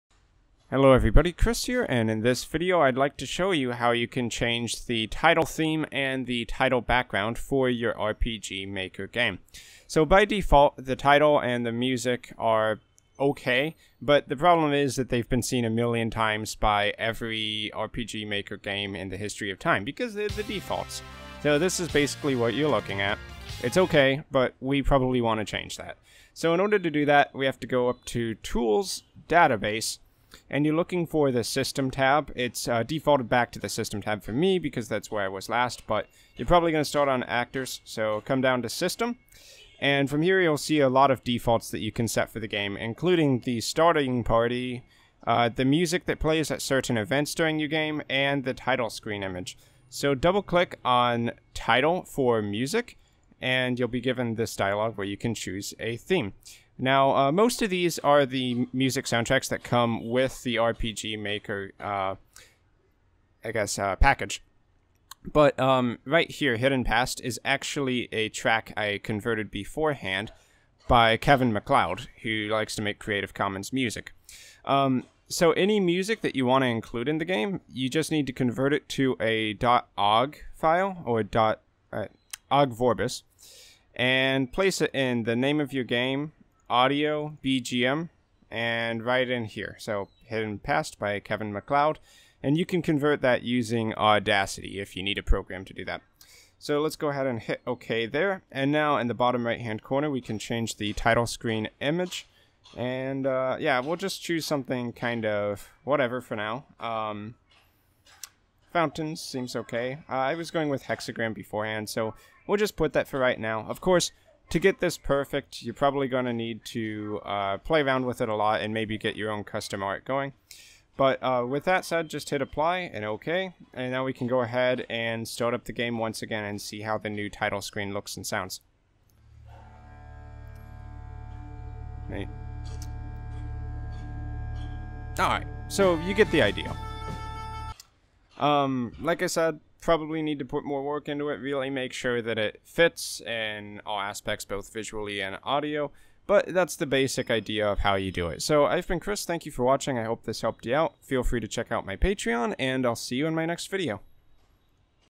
How to Change Main Title Music and Background Image | RPG Maker MV Tutorial
Change your title screen so that it's not the default in RPG Maker MV!

Learn Video Editing - Complete Guide to DaVinci Resolve Course ($5 Off)